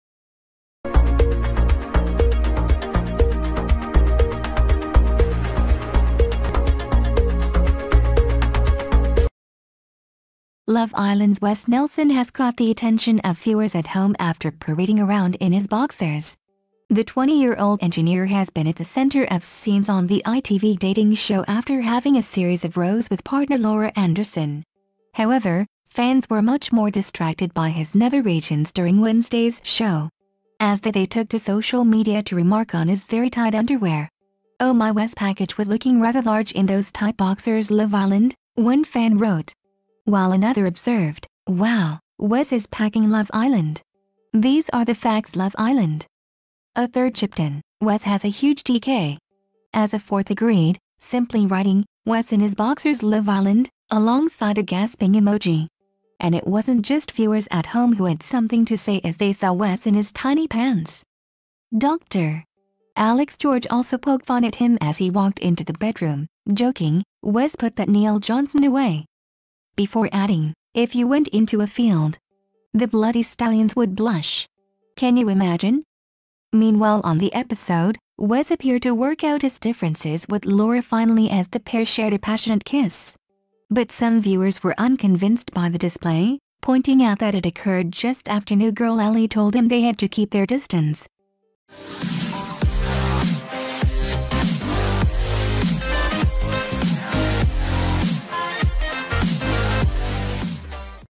Love Island viewers make VERY x-rated discovery about Wes Nelson as they comment on his impressive
Love Island viewers make VERY x-rated discovery about Wes Nelson as they comment on his impressive bulge
Love Island's Wes Nelson has caught the attention of viewers at home after parading around in his boxers.
The 20 year old engineer has been at the centre of scenes on the ITV dating show after having